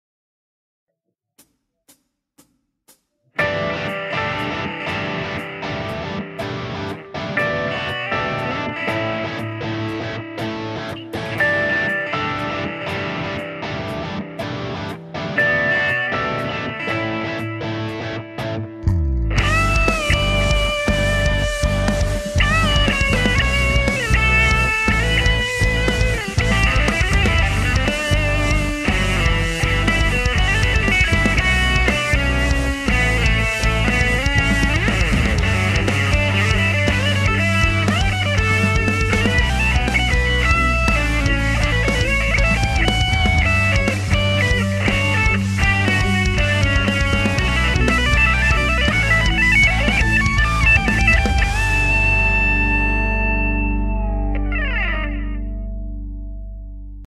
Kiesel Solo Contest 2019 - Kevin Diyco
Here is my entry for the kieselsolocontest2019. The backing track was great to solo over!